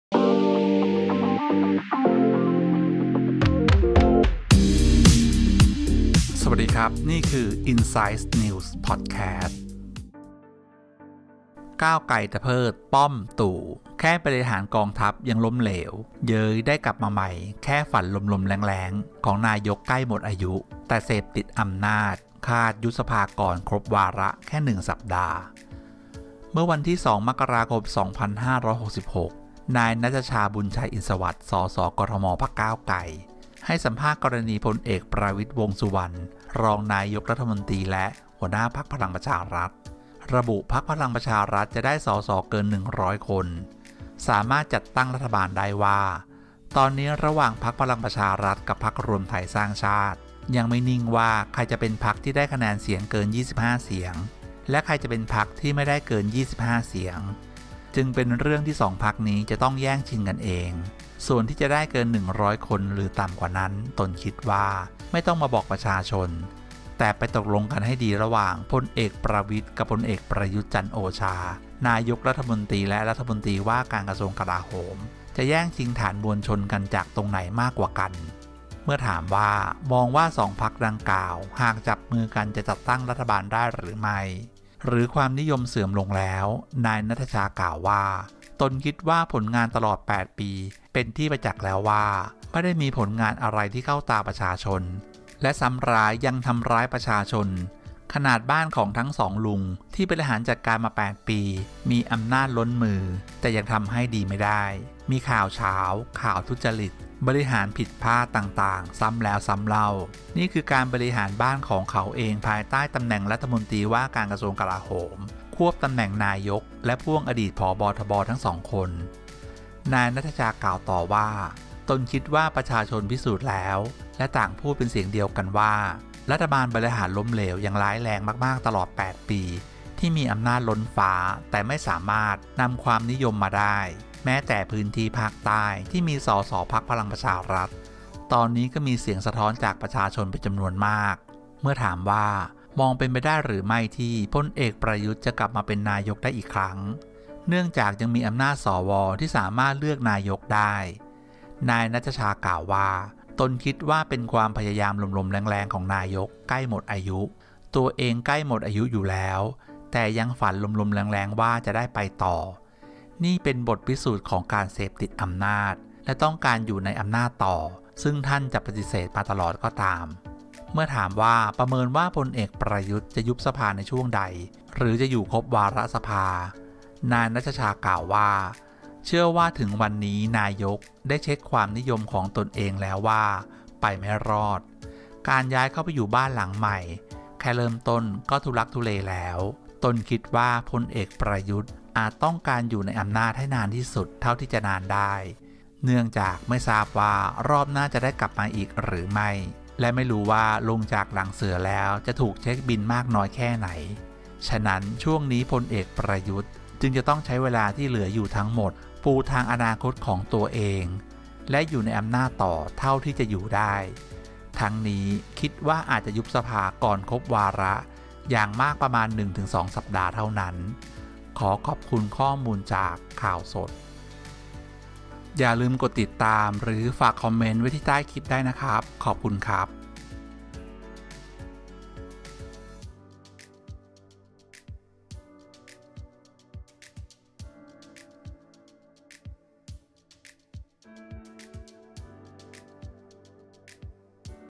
โดนเช็คบิลแน่! "ก้าวไกล" เขย่าขวัญ "ประวิตร - ประยุทธ์" ใกล้หมดอายุ ยังฝันลมๆแล้งๆ|อินไซด์นิวส์ไทย
ก้าวไกล ตะเพิด ‘ป้อม-ตู่’ อย่าฝันลมๆแล้งๆ ได้กลับมาตั้งรัฐบาล ระวังโดนเช็กบิล|อินไซด์นิวส์ไทย
ก้าวไกล ตะเพิด ‘บิ๊กป้อม-บิ๊กตู่’ แค่บริหารกองทัพยังล้มเหลว เย้ยได้กลับมาใหม่ แค่ฝันลมๆ แล้งๆ ของนายกฯ ใกล้หมดอายุ แต่ติดอำนาจ คาดยุบสภาก่อนครบวาระแค่ 1 สัปดาห์
เมื่อวันที่ 2 ม.ค.2566 นายณัฐชา บุญไชยอินสวัสดิ์ ส.ส.กทม. พรรคก้าวไกล (ก.ก.) ให้สัมภาษณ์กรณีพล.อ.ประวิตร วงษ์สุวรรณ รองนายกรัฐมนตรี และหัวหน้าพรรคพลังประชารัฐ (พปชร.) ระบุ พรรคพลังประชารัฐ จะได้ส.ส.เกิน 100 คน สามารถจัดตั้งรัฐบาลได้ว่า ตอนนี้ระหว่างพรรคพลังประชารัฐกับพรรครวมไทยสร้างชาติ (รทสช.) ยังไม่นิ่งว่าใครจะเป็นพรรคที่ได้คะแนนเสียงเกิน 25 เสียงและใครจะเป็นพรรคที่ได้ไม่เกิน 25 เสียง จึงเป็นเรื่องที่ 2 พรรคนี้จะแย่งชิงกันเอง
ส่วนที่จะได้เกิน 100 คน หรือต่ำกว่านั้น ตนคิดว่าไม่ต้องมาบอกประชาชน แต่ไปตกลงกันให้ดีว่าระหว่างพล.อ.ประวิตร กับพล.อ.ประยุทธ์ จันทร์โอชา นายกฯและรมว.กลาโหม จะแย่งชิงฐานมวลชนกันจากตรงไหนมากกว่ากัน
เมื่อถามว่ามองว่า 2 พรรคดังกล่าว หากจับมือกันจะจัดตั้งรัฐบาลได้หรือไม่ หรือความนิยมเสื่อมลงแล้ว
นายณัฐชา กล่าวว่า ตนคิดว่าผลงานตลอด 8 ปีเป็นที่ประจักษ์แล้วว่า ไม่ได้มีผลงานอะไรที่เข้าตาประชาชน และซ้ำร้ายยังทำร้ายประชาชน ขนาดบ้านของทั้ง 2 ลุงที่บริหารจัดการมา 8 ปี มีอำนาจล้นมือแต่ยังทำให้ดีไม่ได้ มีข่าวฉาว ข่าวทุจริต บริหารผิดพลาดต่างๆ ซ้ำแล้วซ้ำเล่า นี่คือการบริหารบ้านของเขาเองภายใต้ตำแหน่งรมว.กลาโหม ควบตำแหน่งนายกฯ และพ่วงอดีตผบ.ทบ. ทั้ง 2 คน
นายณัฐชา กล่าวต่อว่า ตนคิดว่าประชาชนพิสูจน์แล้ว และต่างพูดเป็นเสียงเดียวกันว่า รัฐบาลบริหารล้มเหลวอย่างร้ายแรงมากๆ ตลอด 8 ปีที่มีอำนาจล้นฟ้าแต่ไม่สามารถนำความนิยมมาได้ แม้แต่พื้นที่ภาคใต้ ที่มีส.ส.พรรคพลังประชารัฐ ตอนนี้ก็มีเสียงสะท้อนจากประชาชนเป็นจำนวนมาก
เมื่อถามว่ามองว่าเป็นไปได้หรือไม่ที่พล.อ.ประยุทธ์ จะกลับมาเป็นนายกฯได้อีกครั้ง เนื่องจากยังมีอำนาจส.ว.ที่สามารถเลือกนายกฯได้
นายณัฐชา กล่าวว่า ตนคิดว่าเป็นความพยายามลมๆ แล้งๆ ของนายกฯใกล้หมดอายุ ตัวเองใกล้หมดอายุอยู่แล้ว แต่ยังฝันลมๆ แล้งๆ ว่าจะไปต่อ นี่เป็นบทพิสูจน์ของการติดอำนาจ และต้องการอยู่ในอำนาจต่อ ซึ่งท่านจะปฏิเสธมาตลอดก็ตาม
เมื่อถามว่าประเมินว่าพล.อ.ประยุทธ์ จะยุบสภาในช่วงใดหรือจะอยู่ครบวาระสภา
นายณัฐชา กล่าวว่า เชื่อว่าถึงวันนี้นายกฯได้เช็กความนิยมของตนเองแล้วว่าไปไม่รอด การย้ายเข้าไปอยู่บ้านหลังใหม่ แค่เริ่มต้นก็ทุลักทุเลแล้ว ตนคิดว่าพล.อ.ประยุทธ์ อาจต้องการอยู่ในอำนาจให้นานที่สุดเท่าที่จะนานได้ เนื่องจากไม่ทราบว่ารอบหน้าจะได้กลับมาอีกหรือไม่ และไม่รู้ว่าลงจากหลังเสือแล้ว จะถูกเช็คบิลมากน้อยแค่ไหน
ฉะนั้น ช่วงนี้พล.อ.ประยุทธ์ จึงจะใช้เวลาที่เหลืออยู่ ทั้งหมดปูทางอนาคตของตนเอง และอยู่ในอำนาจต่อเท่าที่อยู่ได้ ทั้งนี้คิดว่าอาจจะยุบสภาก่อนครบวาระอย่างมากประมาณ 1-2 สัปดาห์เท่านั้น

ข่าวสด khaosod